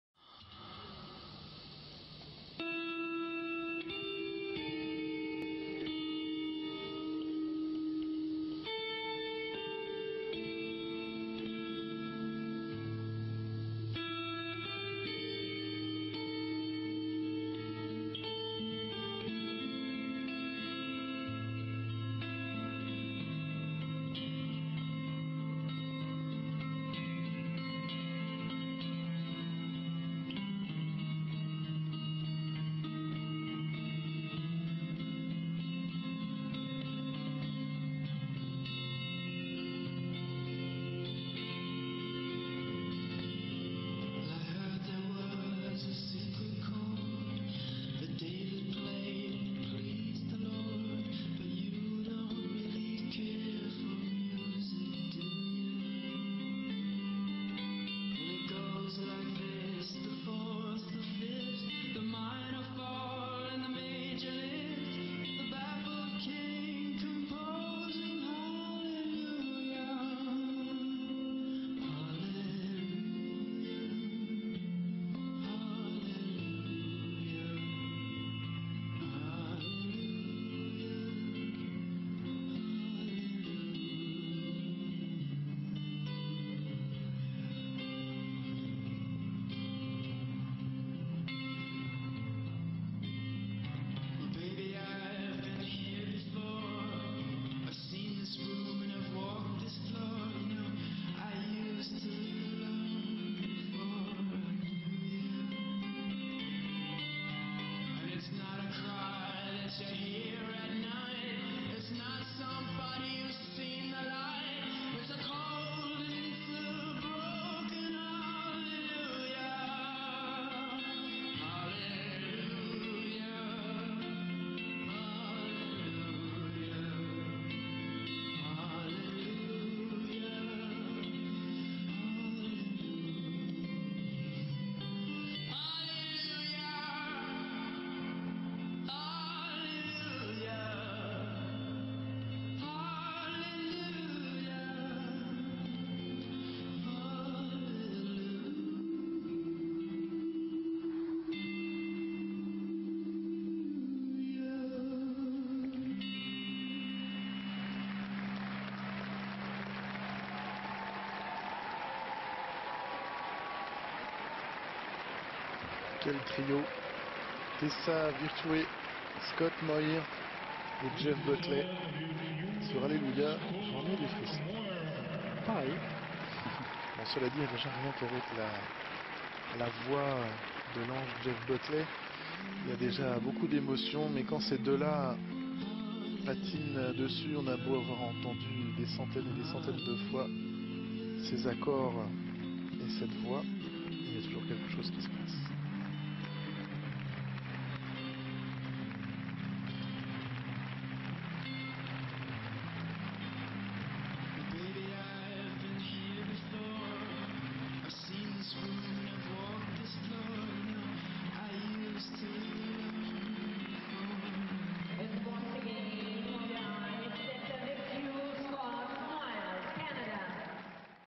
Tessa Virtue - Scott Moir (CAN) / Ex #1 / CoR 2012 [FHD] (ES) [+comm]
Tessa VIRTUE - Scott MOIR (CAN) / Exhibition Gala #1 / Figure Skating: Ice Dance / ISU Grand Prix of Figure Skating: Cup of Russia (Rostelecom Cup) - 2012/2013, Moscow.